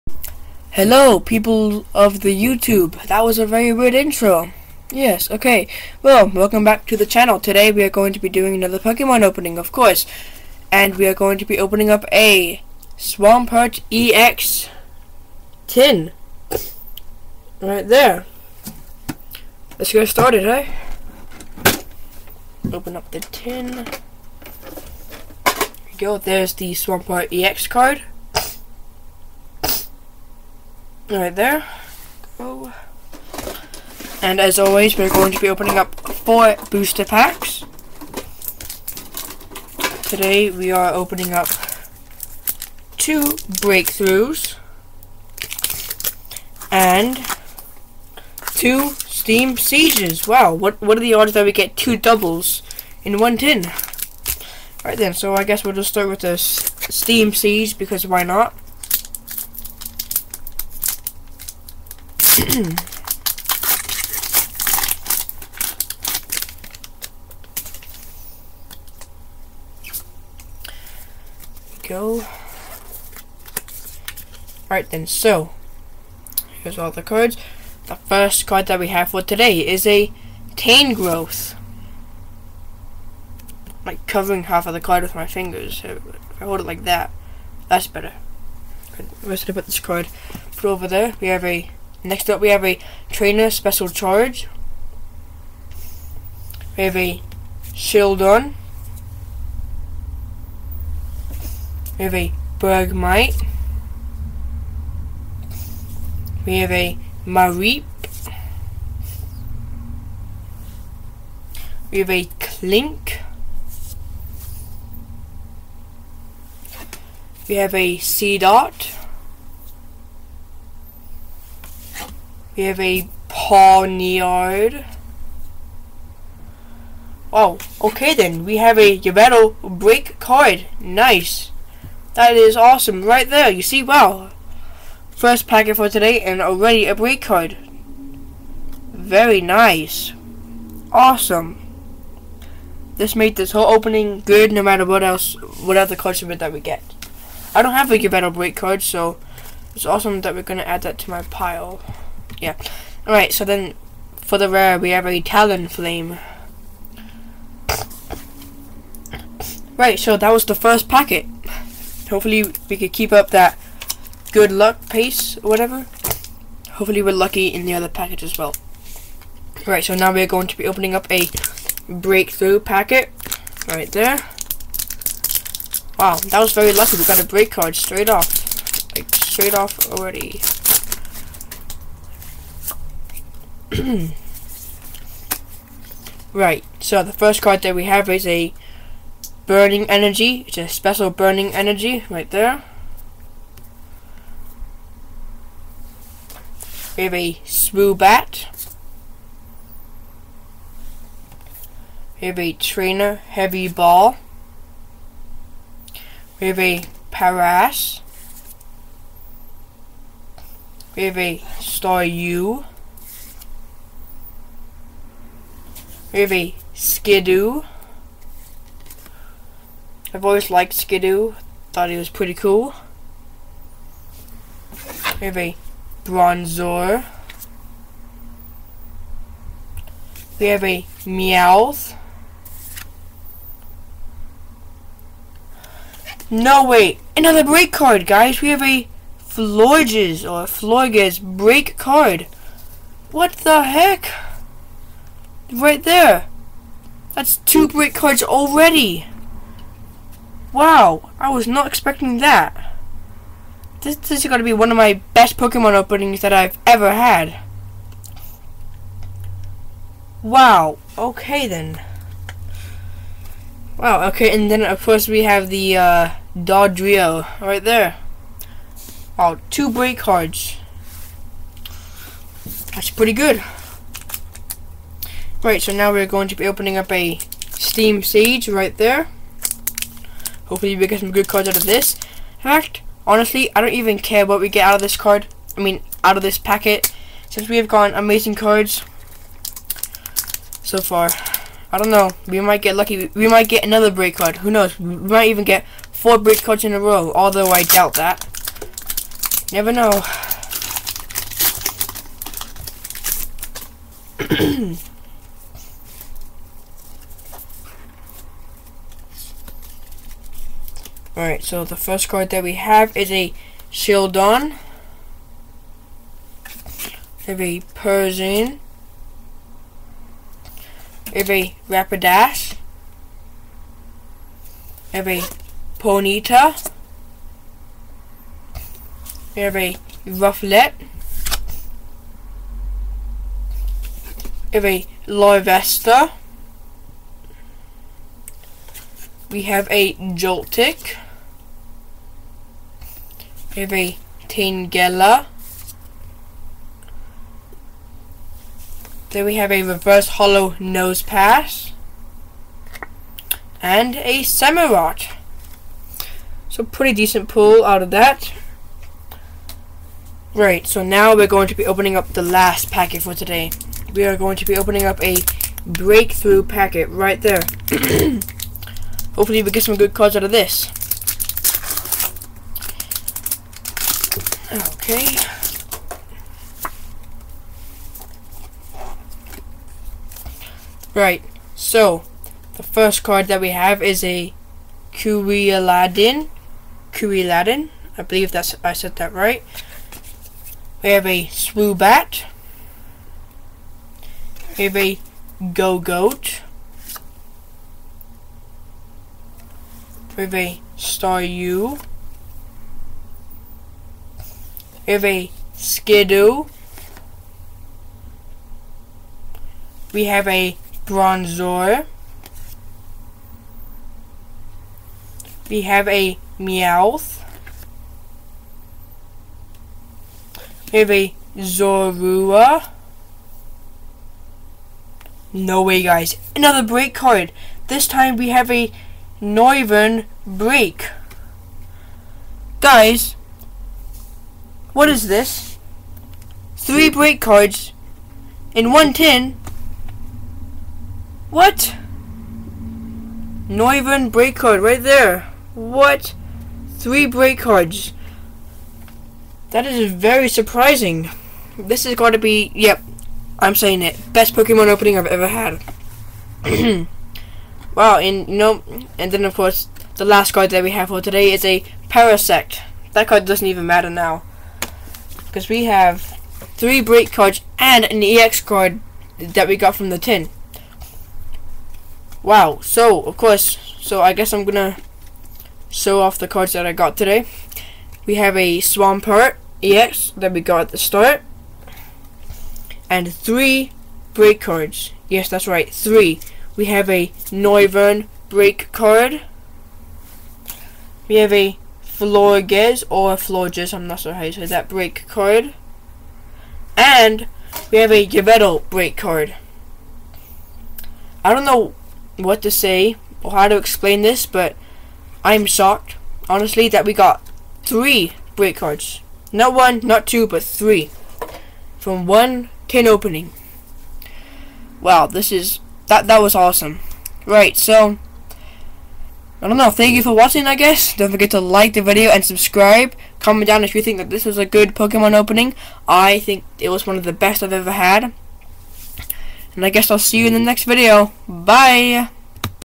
MOST BREAK CARDS EVER! Swampert tin opeing!
Today we are going to be doing another pokemon opening!